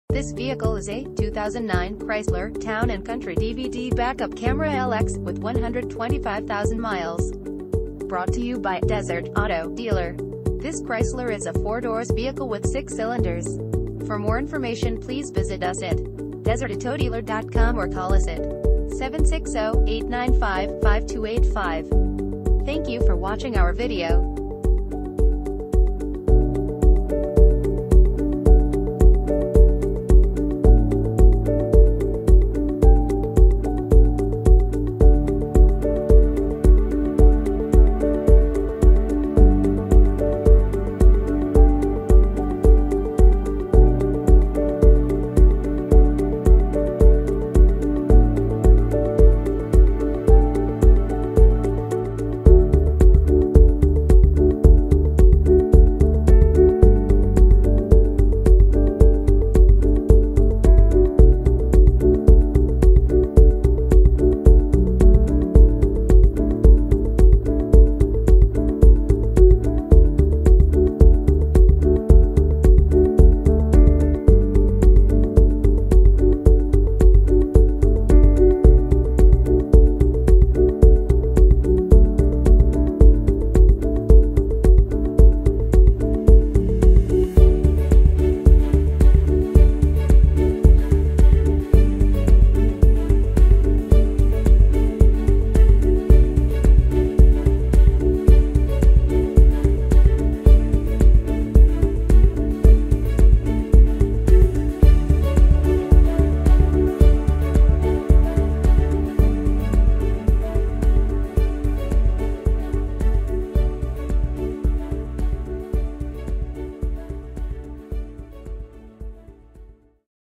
2009 Chrysler Town Country DVD Back-Up Camera - Desert Auto Dealer
2009 Chrysler  Town  Country DVD Back-Up Camera

Clean title, Extended Warranty Available, 125,000 miles, Automatic, Electric, FWD: Front Wheel Drive, 4 doors, 6 cylinders, Gasoline, MPG City 17 and 24 in Hwy.